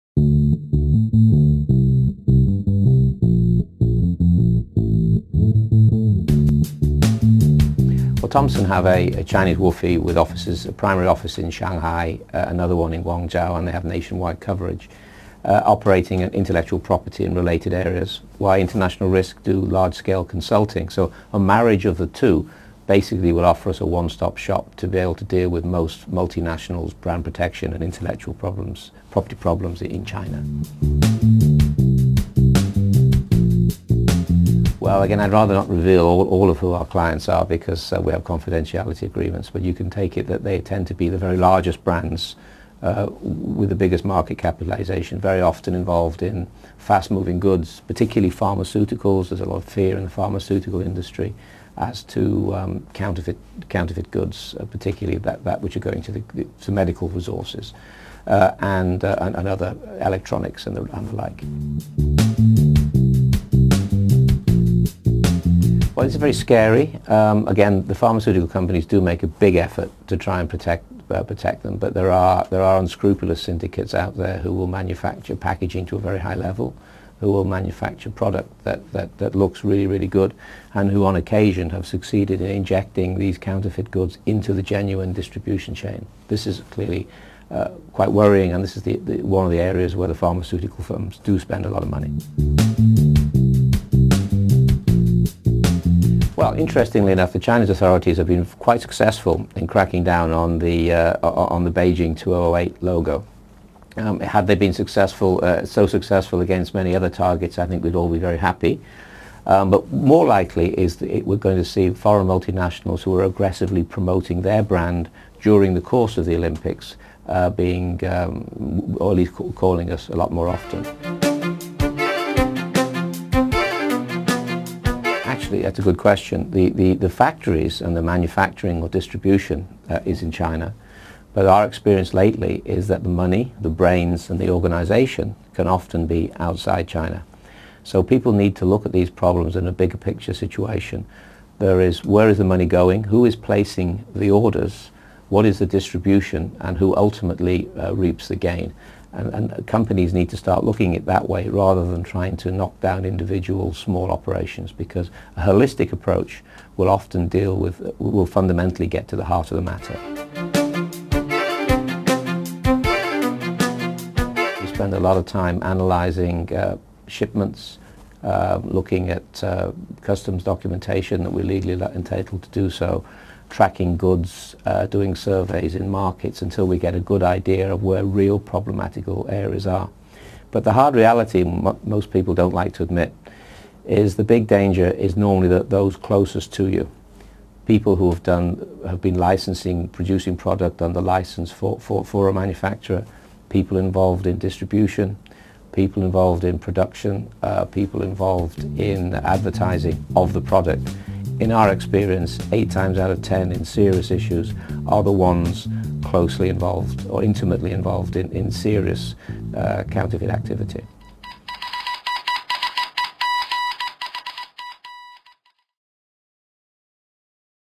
Acquisition seals mainland entry
Steve Vickers is the president and chief executive of International Risk, a company that deals with business investigation and brand protection.